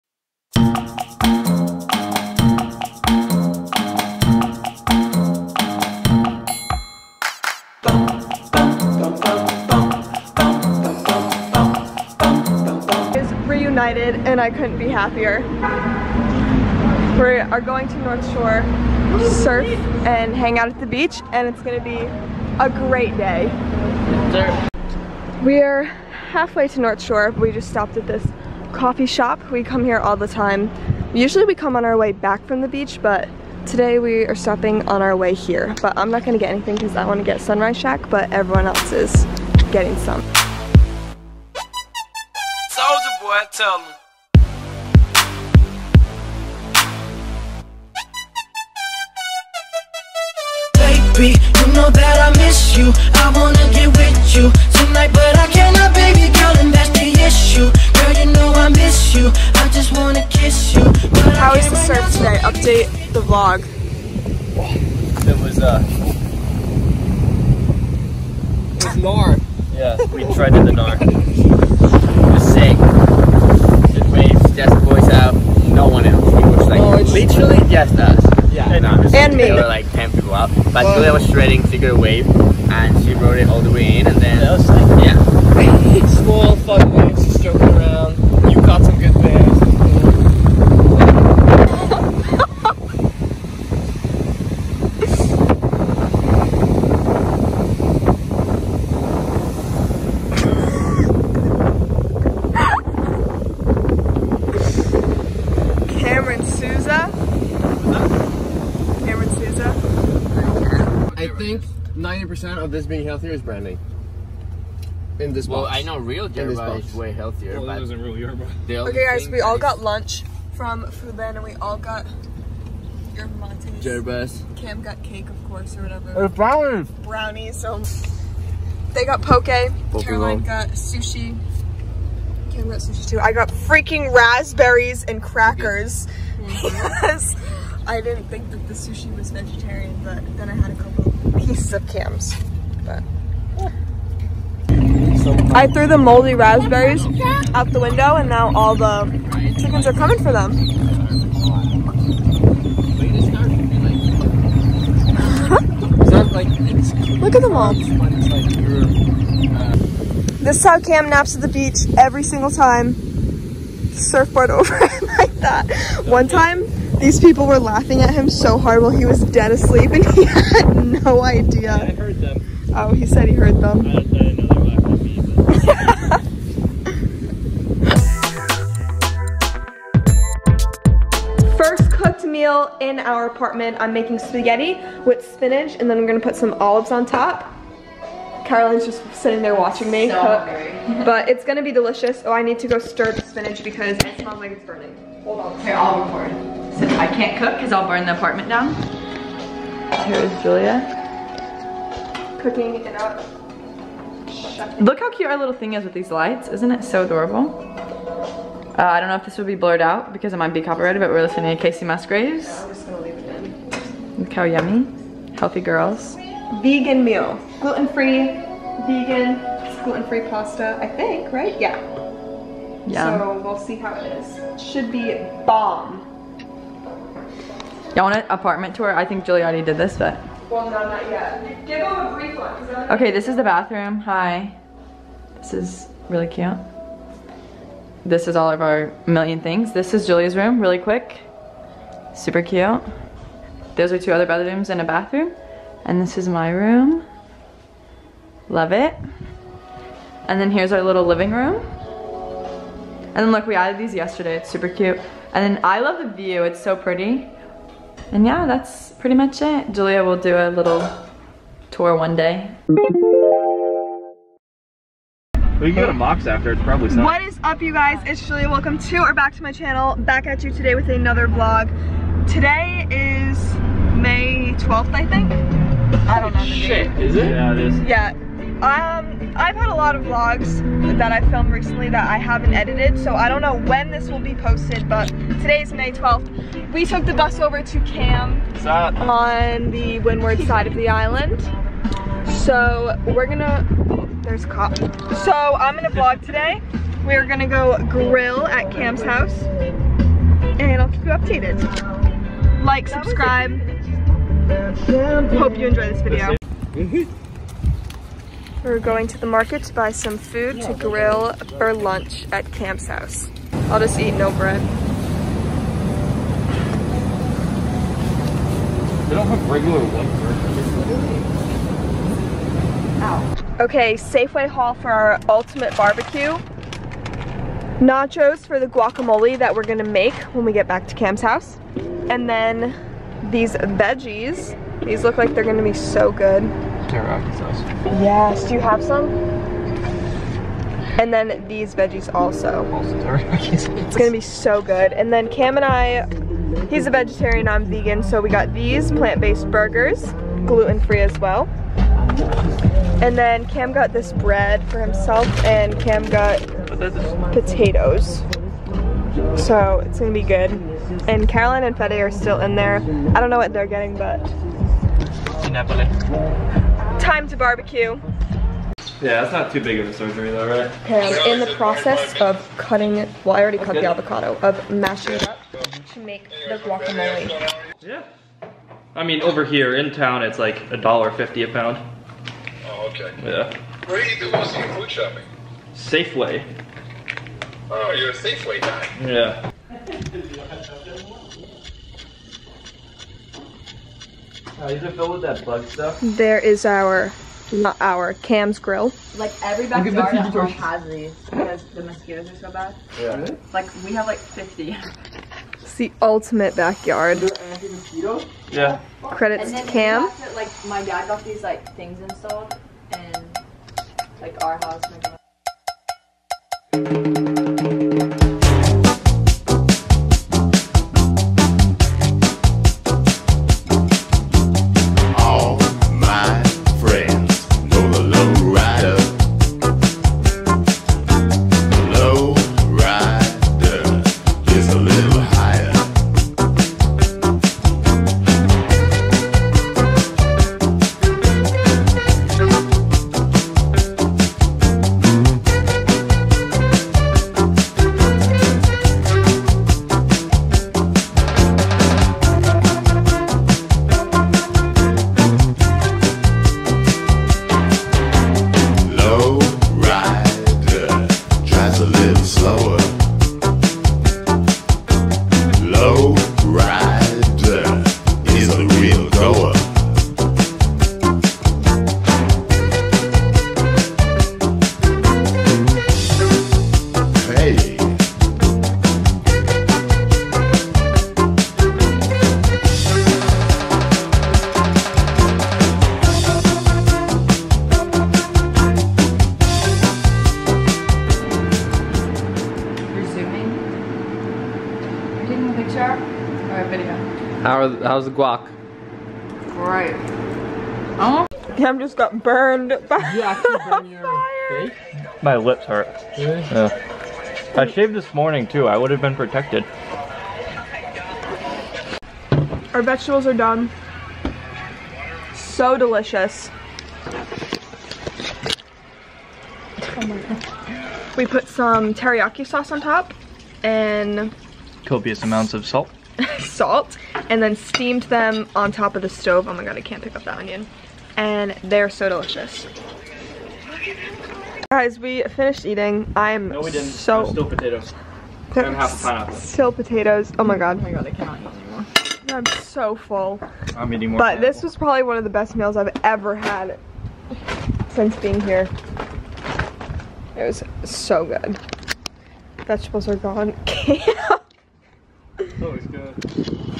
SURFING, MINI APARTMENT TOUR, & COOKOUT AT CAMS
I am beyond grateful for you all💙💙